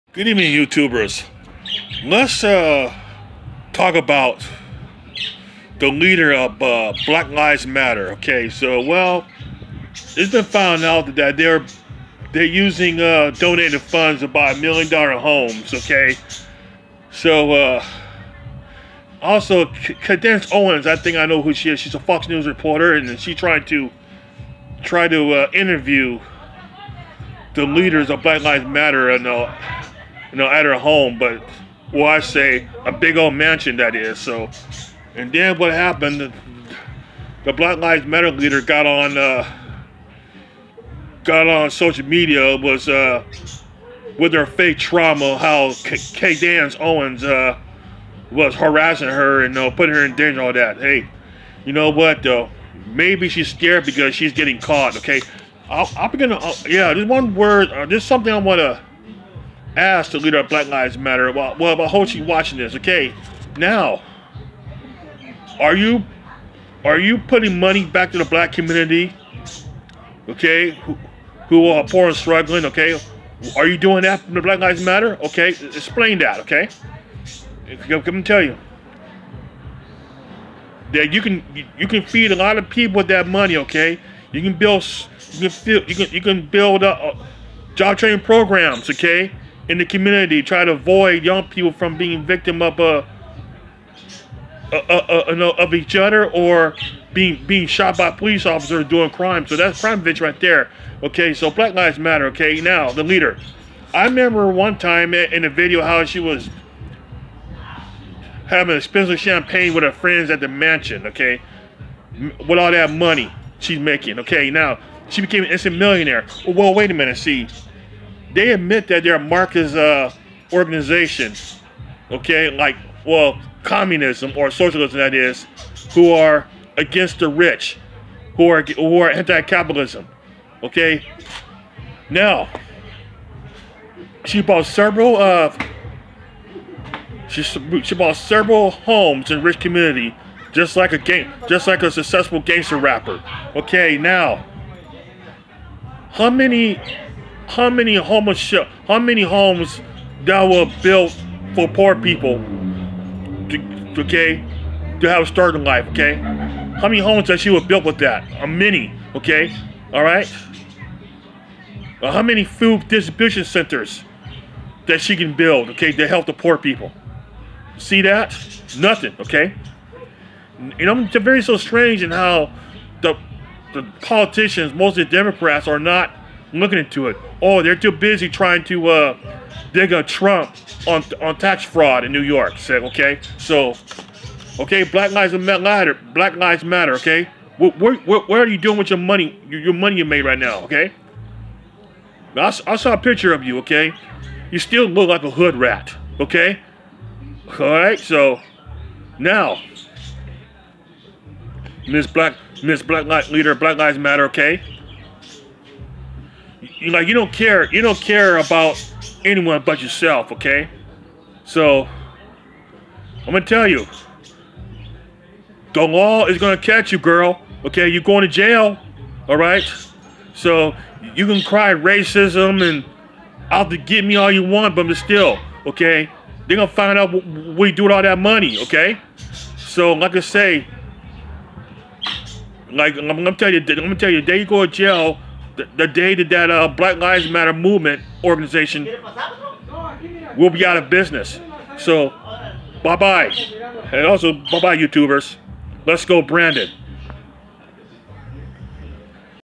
Leader of Black Lives Matter. Where's the money you made. You be in jail soon.
BLM is a fraud